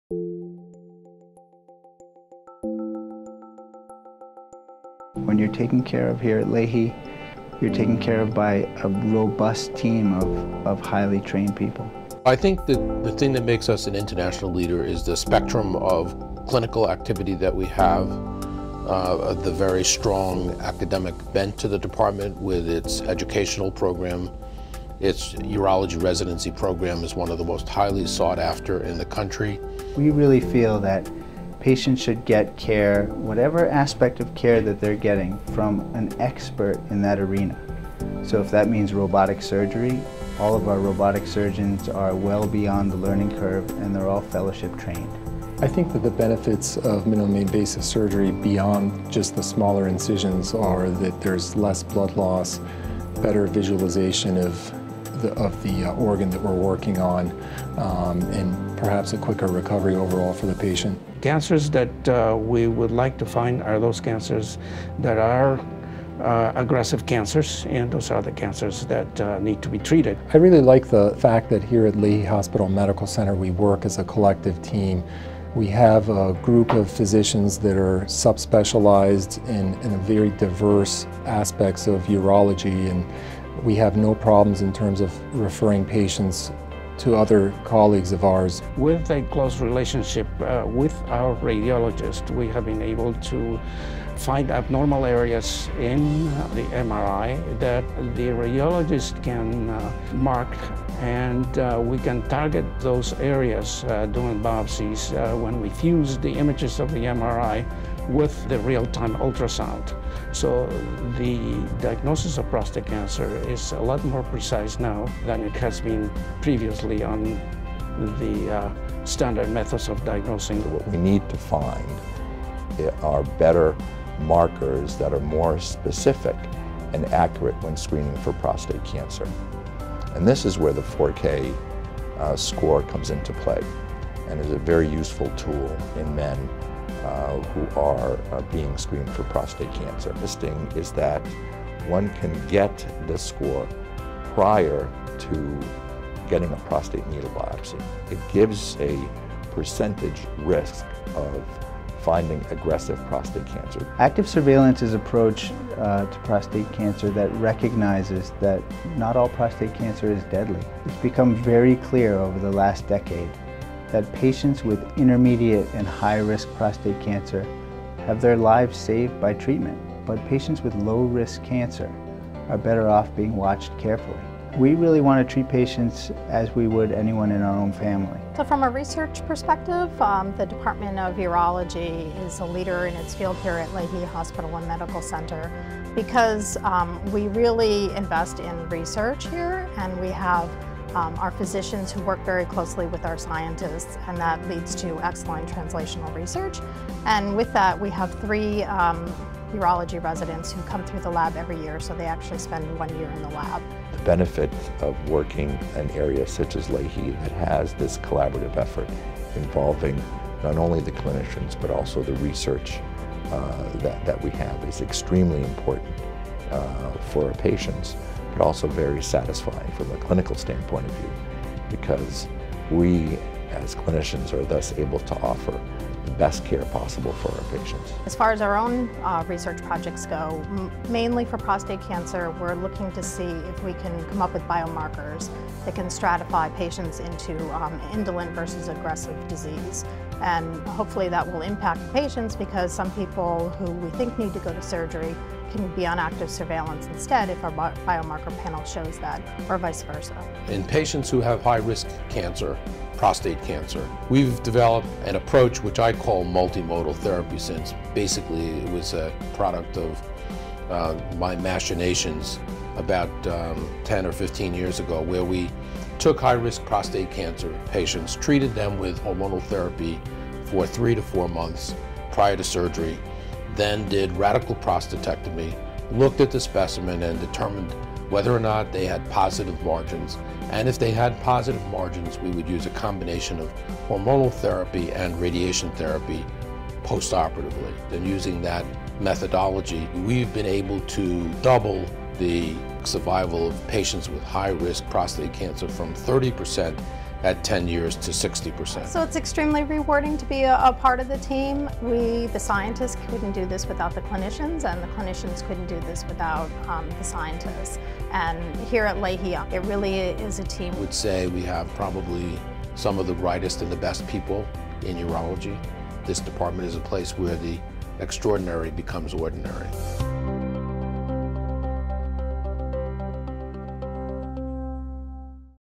Lahey Hospital and Medical Center, Institute of Urology
Lahey Hospital and Medical Center's Urology Department is recognized for both clinical and academic excellence. The team of pre-eminent urologists work together to develop customized treatment plans for patients, ensuring the best possible outcomes. Lahey Hospital & Medical Center is at the leading edge of prostate cancer treatment.

Lahey offers innovative, state of the art care to their patients. The urology group surrounds patients with a multidisciplinary group of specialists. Lahey urologist have led the way in developing open and minimally invasive treatments for prostate cancer, dramatically improving the quality of life for those who undergo treatment for the disease.